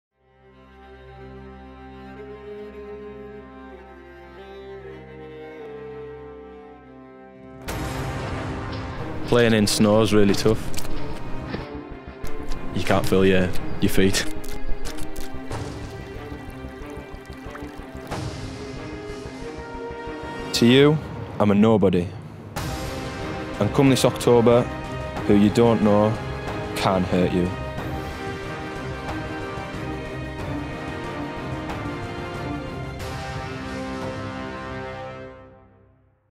Brett Phillips - Behind the Scenes
Scotland will look to build on their 2016 Four Nations experience leading into Rugby League World Cup 2017.